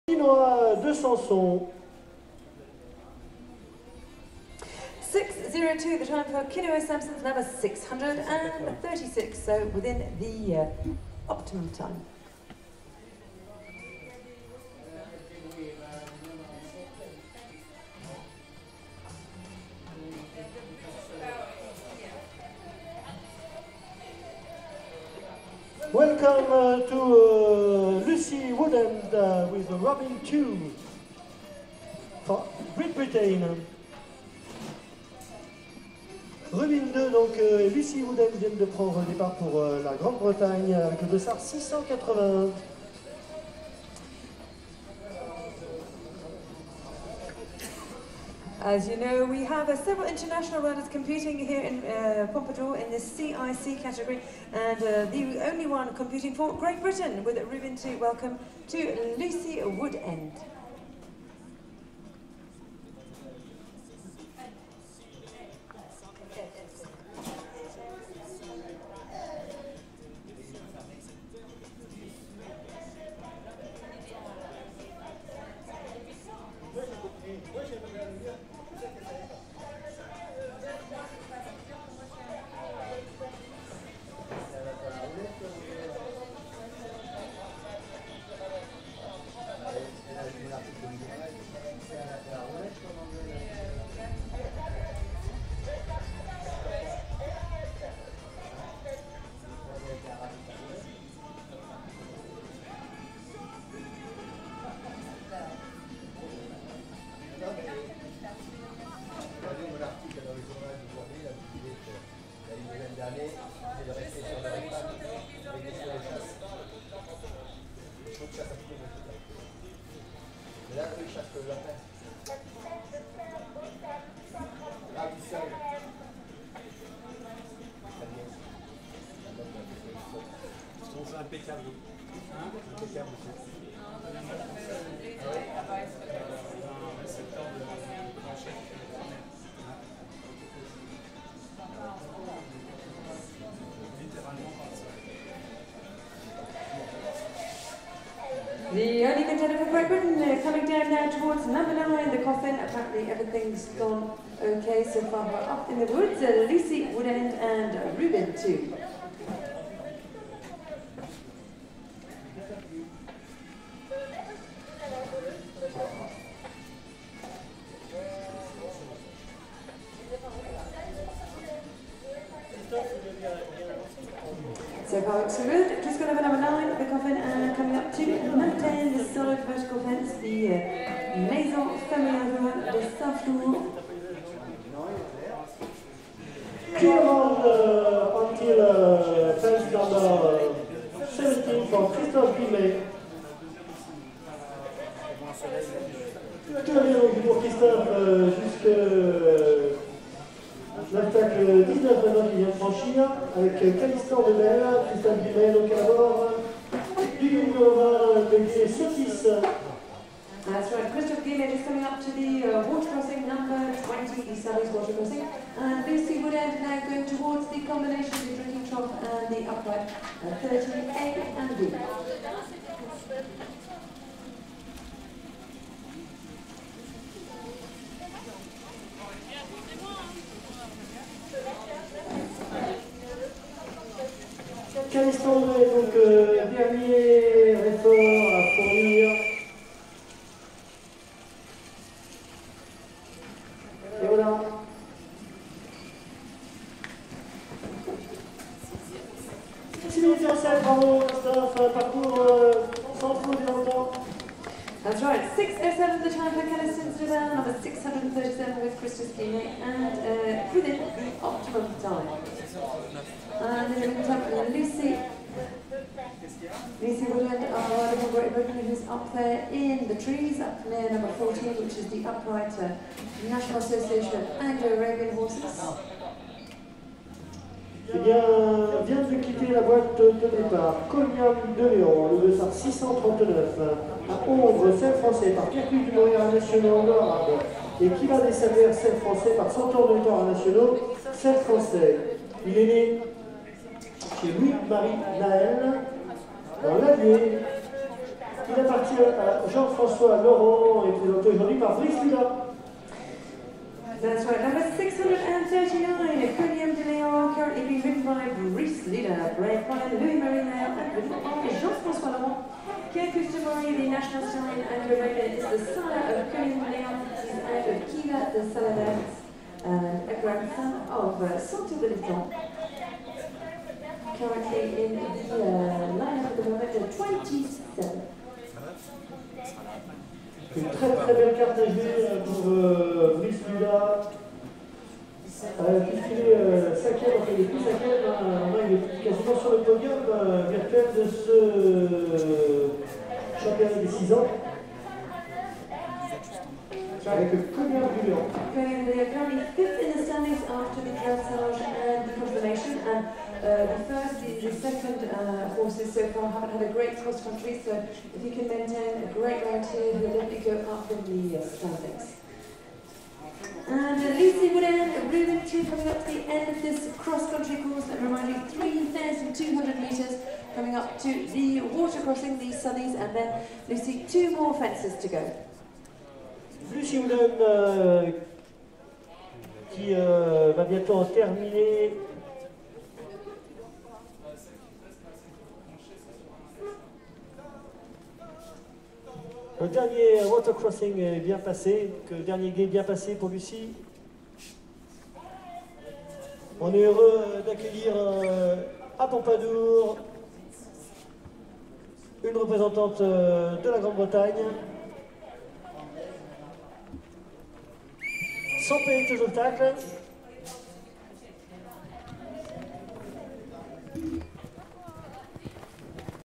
Rubin II Pompadour 2010 CIC* XC
Rubin going XC in the 6yo CIC* La Grande Semaine de Pompadour 2010